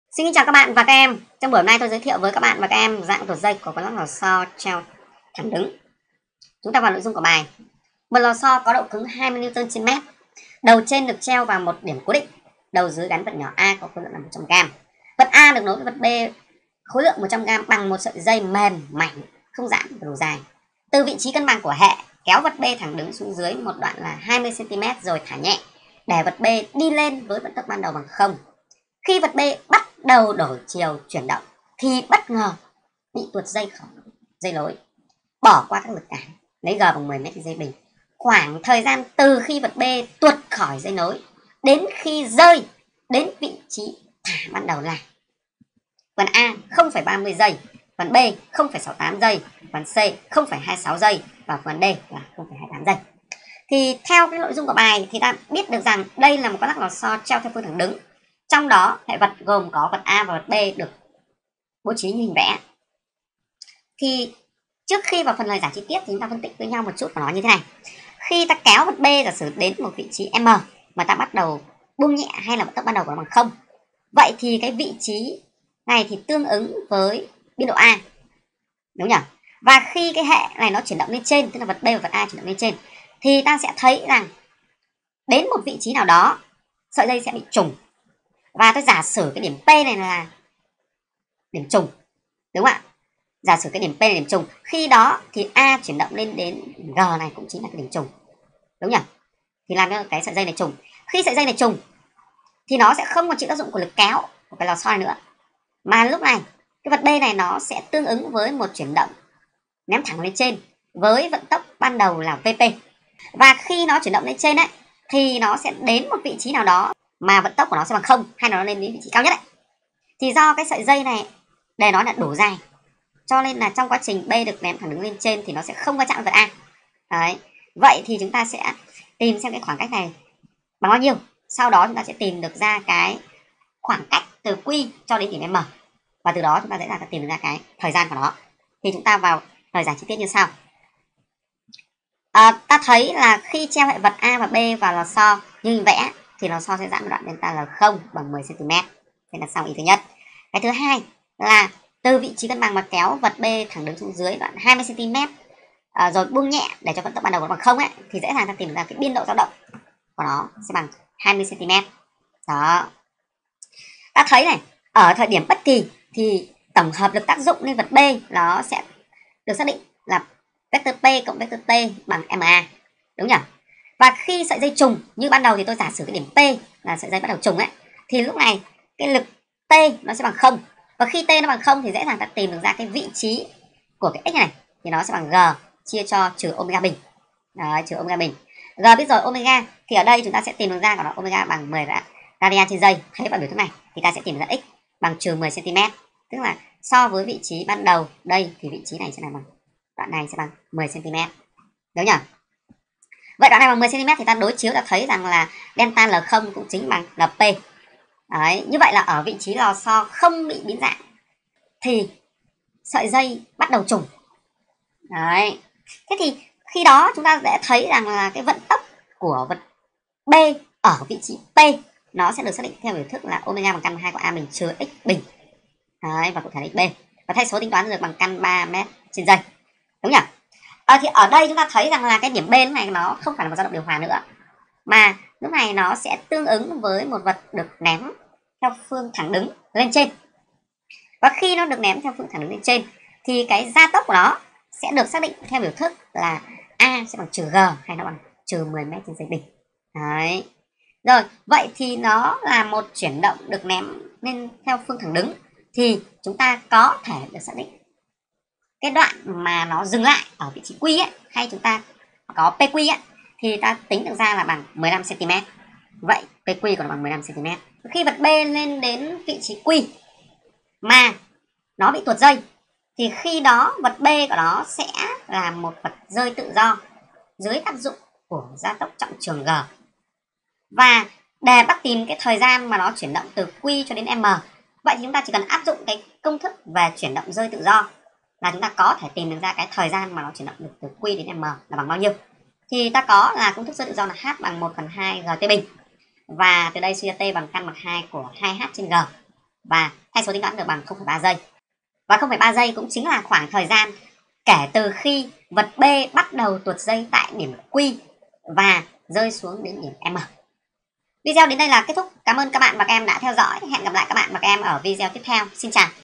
Tăng Giáp: Dạng tuột dây của con lắc lò xo treo thẳng đứng
Dạng tuột dây của con lắc lò xo treo thẳng đứng là dạng toán nâng cao. Tôi xin giới thiệu nội dung

Một lò xo có độ cứng 20N/m, đầu trên được treo vào một điểm cố định, đầu dưới gắn vật nhỏ A có khối lượng 100g, vật A được nối với vật B khối lượng 100g bằng môt sợi dây mềm, mảnh, không dãn và đủ dài. Từ vị trí cân bằng của hệ, kéo vật B thẳng đứng xuống dưới một đoạn 20cm rồi thả nhẹ để vật B đi lên với vận tốc ban đầu bằng không. Khi vật B bắt đầu đổi chiều chuyển động thì bất ngờ bị tuột khỏi dây nối. Bỏ qua các lực cản, lấy g = 10m/s2. Khoảng thời gian từ khi vật B tuột khỏi dây nối đến khi rơi đến vị trí thả ban đầu là:
A.  0,30  s
B. 0,68  s
C. 0,26  s
D. 0,28  s